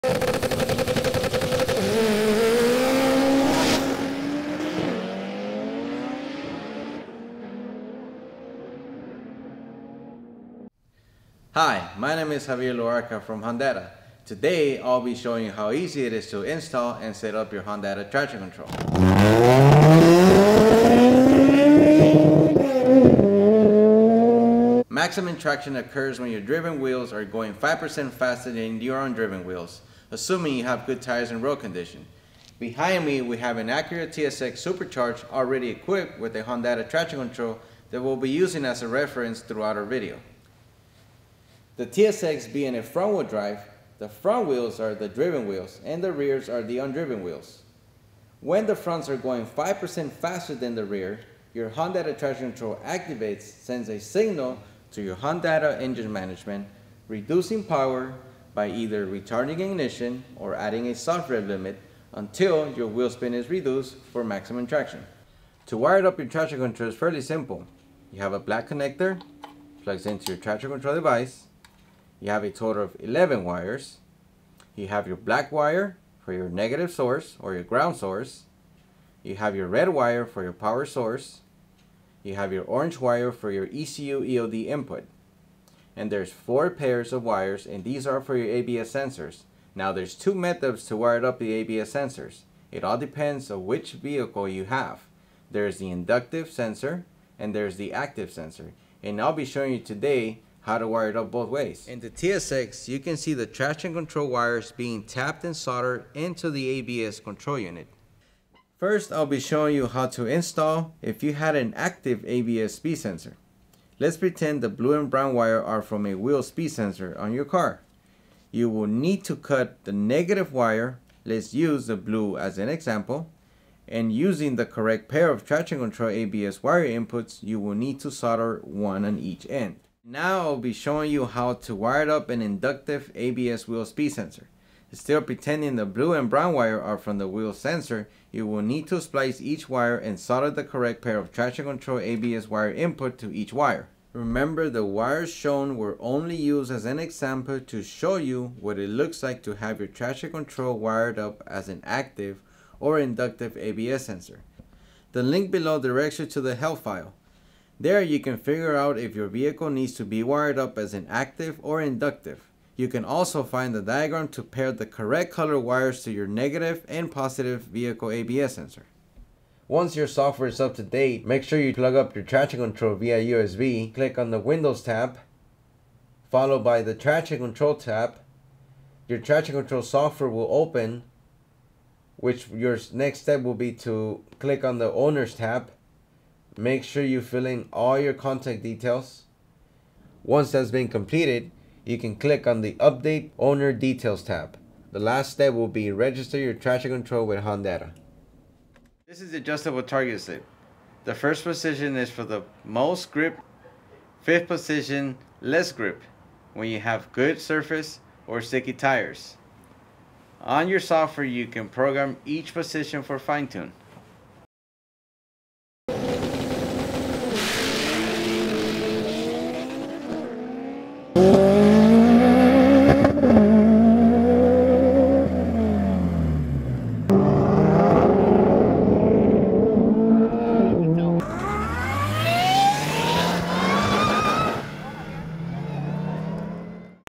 Hondata Traction Control Install
D.I.Y/How the Hondata Traction Control works.